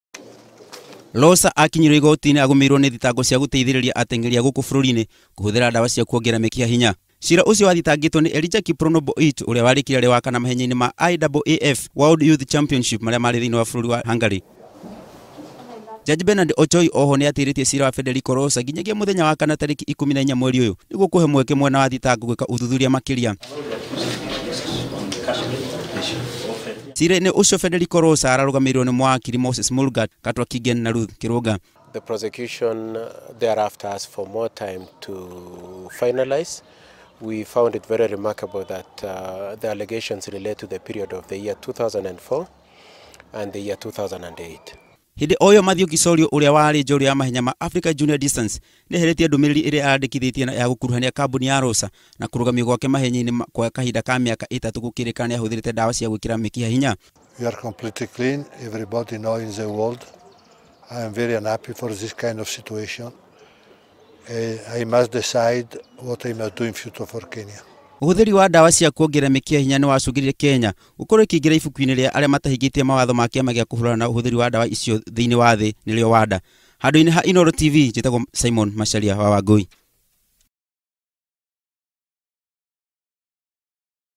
Federico Rosa Kurekererio na bodi ya 300,000
Murugamiriri wa Rosa Associate Federico Rosa  uria  niwe murugamiriri wa  atengéri amwe a guku bururi-ini niarekereirio na bodi ya shillingi ngiri magana matatu ni igoti ria Kibera guku mucii wa Nairobi na oho kurio aneane passport yake kuri igoti